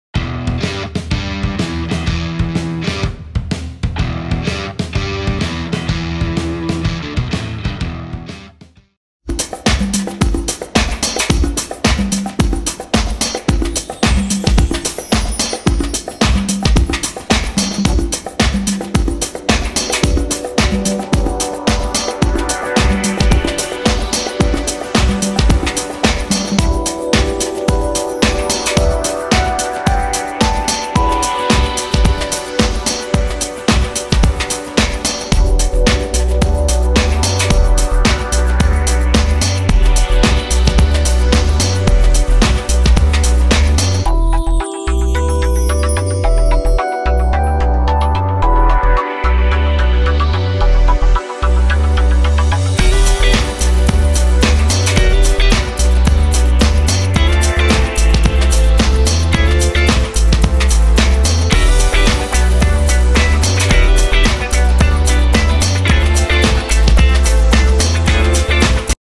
OC-TOBER Day 1: An OC With Long Hair
This is the first drawing for my OC-Tober 2023 drawing challenge. For this first prompt, I chose to draw a teenage cyborg girl with long hair. The cyborg’s name is Lucille Bliss. Don’t you agree that she enjoys the season of fall?

This picture can also be found on my DeviantArt homepage.

This picture was drawn on IbisPaintX and this video was edited on PowerDirector.